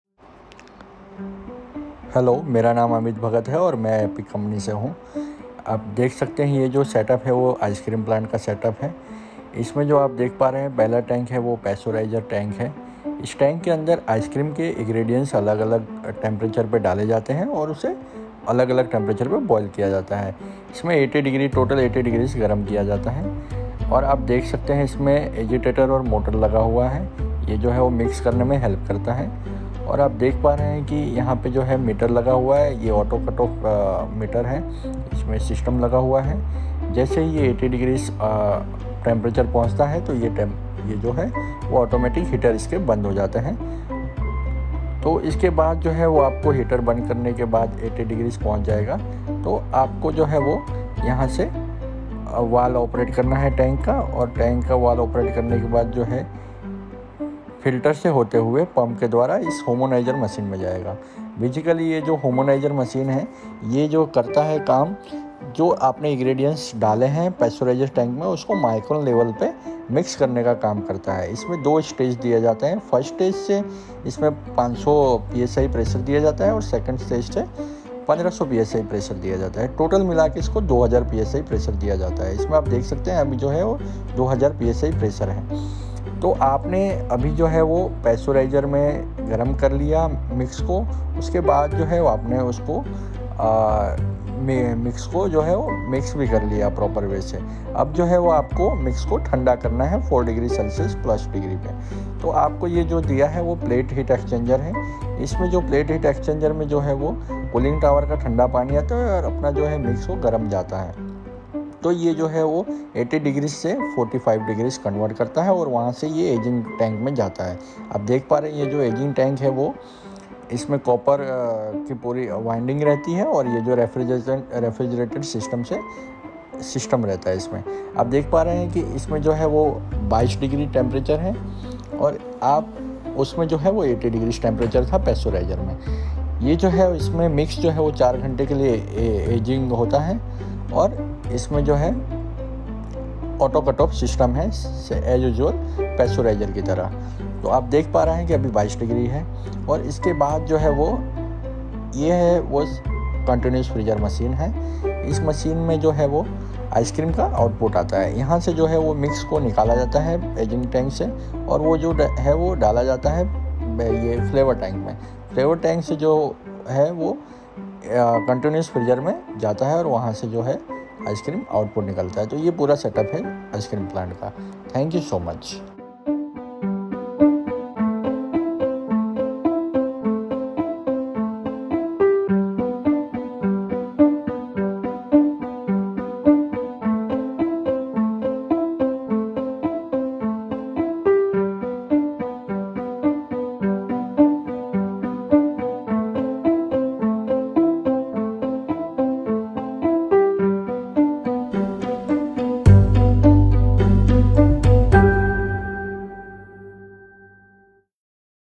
Ice cream machine ice cream plant full description video how to operate ice cream plant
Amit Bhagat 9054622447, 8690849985
Ice cream plant Epic Visio & technology / Epic Dairy Technology Ahmedabad Gujarat manufacture This ice cream is located at Nagpur Maharashtra Logic Ice Cream.